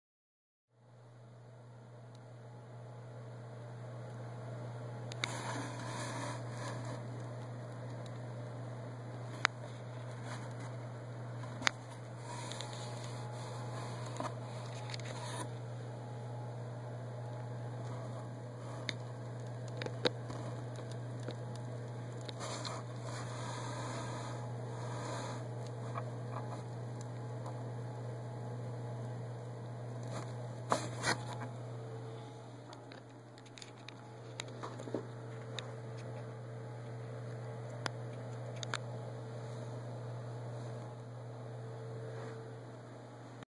Black widow baby
Found this black widow in a closet at the office.